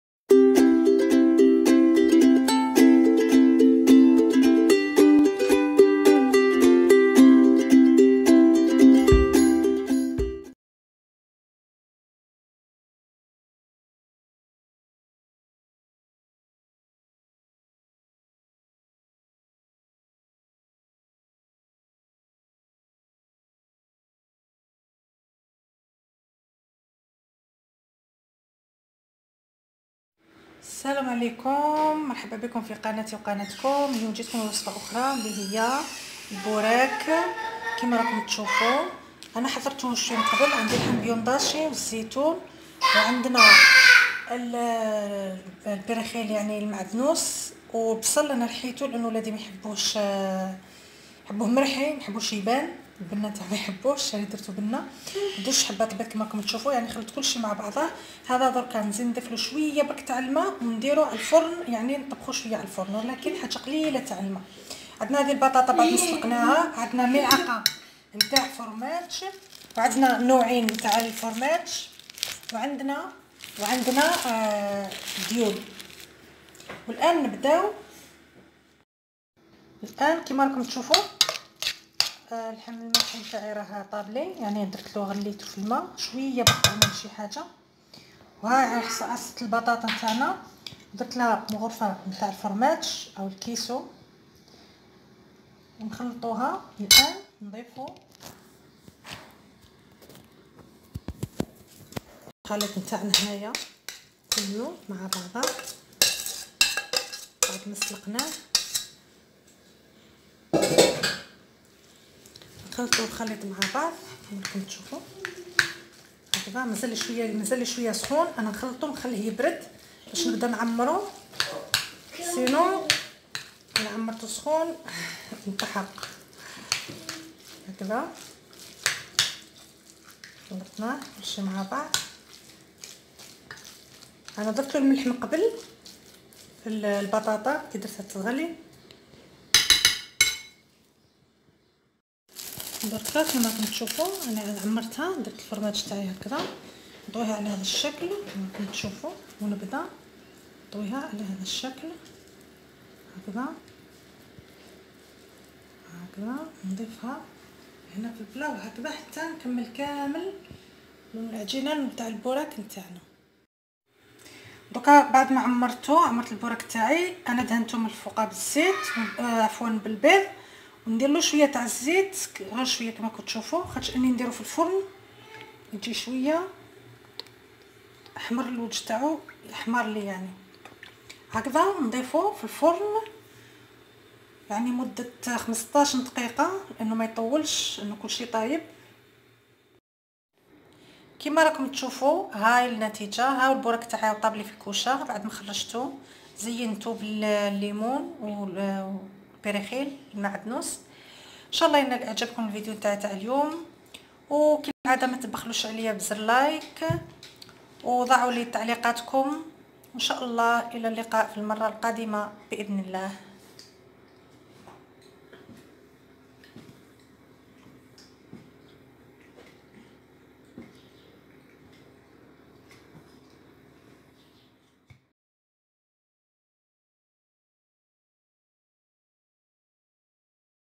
بوراك عالفرن بنين وساهل شهي 😋👍ويعطي نظرة شابة على مائدة رمضان
بوراك عالفرن لام مريم ومحمد